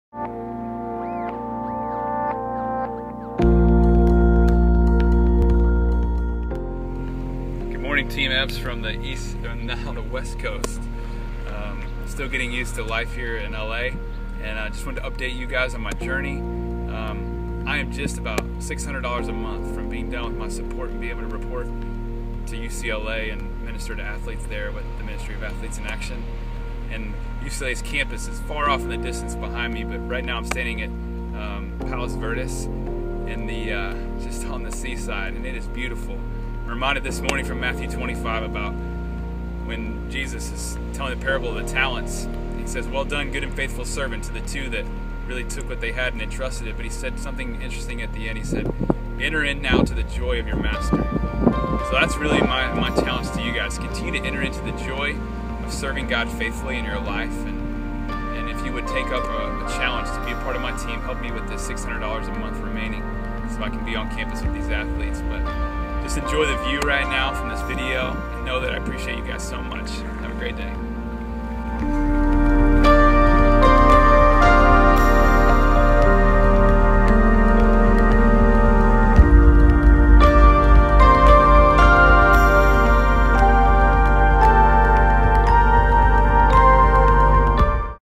Team Eppes Support Update - Nov. 4, 2013
From Palos Verdes, CA I give an update as to how close I am to starting at UCLA with Athletes in Action.  Visit the newly redesigned TeamEppes.com for pictures and sounds from what God is doing in my ministry and its there you can set up your giving as well.